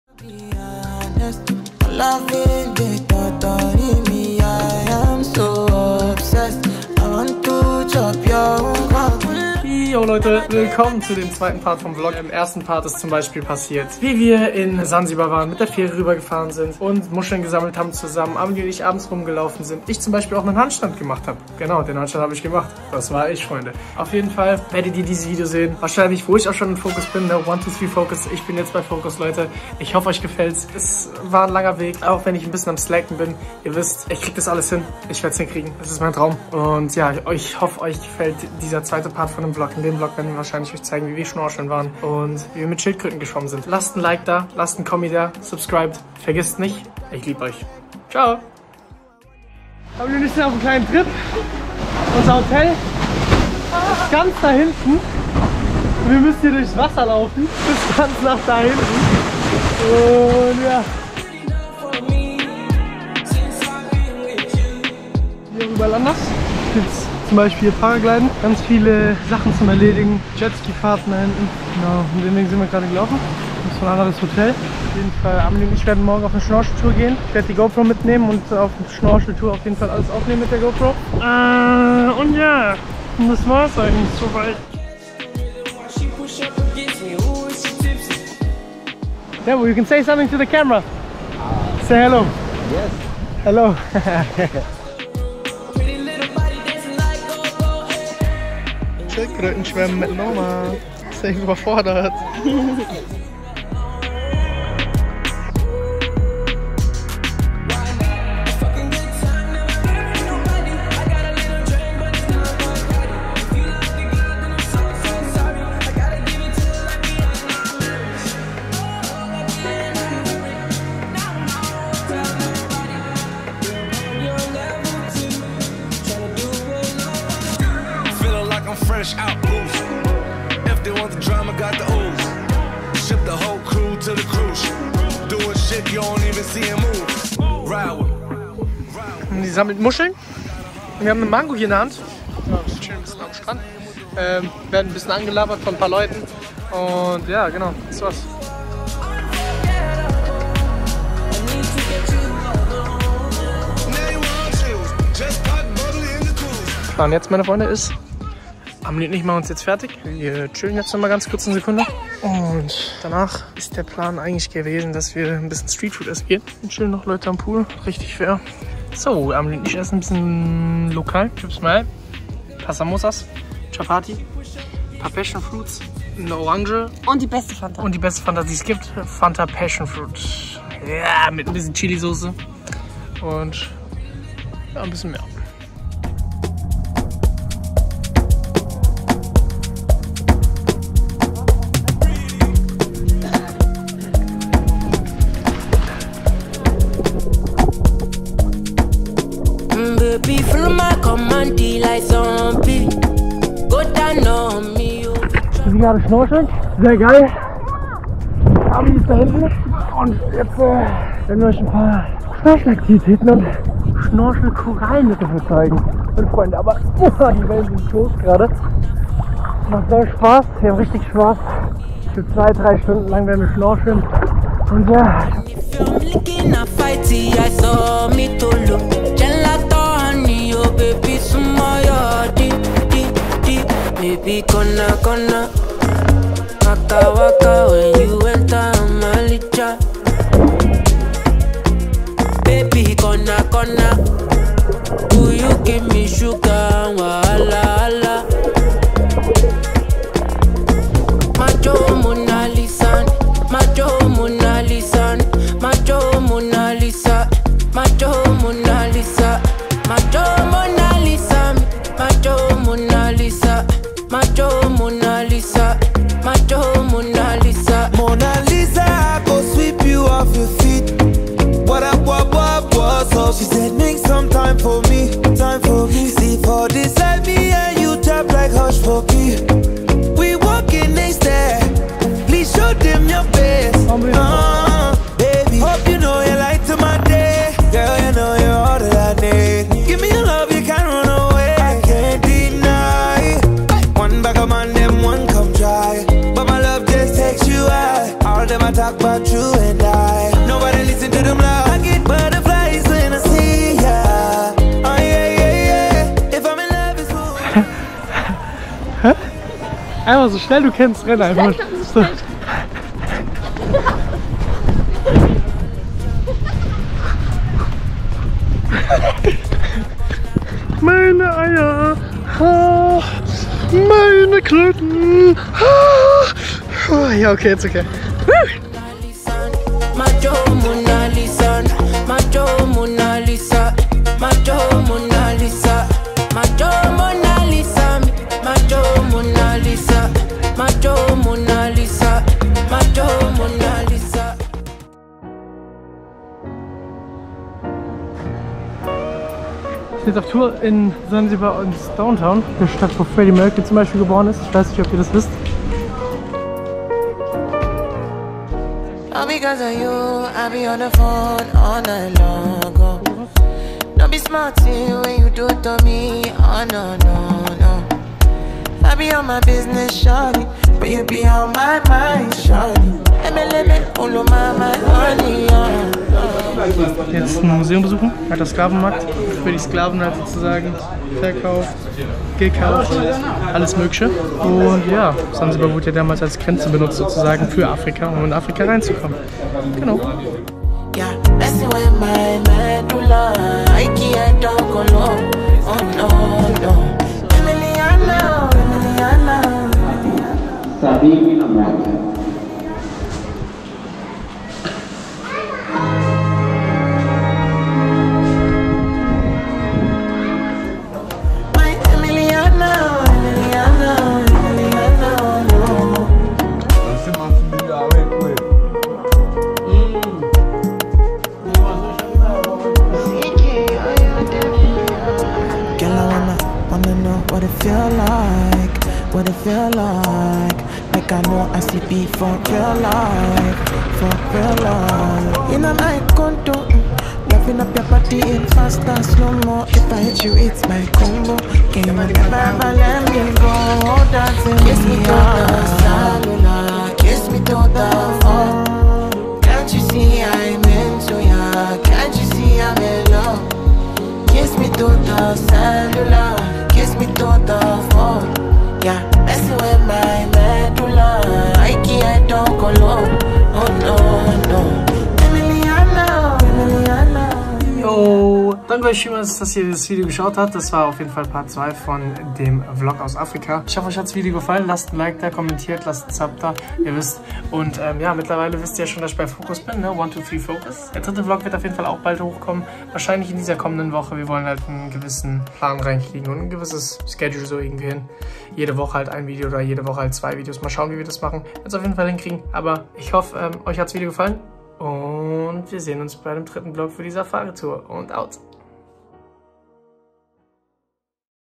Wir tauchen mit Schildkröten!? 🐢 | Tansania Vlog #2 | Lame
In diesem Video seht ihr den zweiten Teil unserer Reise nach Tansania, in dem wir unter anderem mit Schildkröten tauchen. Wenn euch das Video gefällt, lasst gerne ein Like und ein Abo da!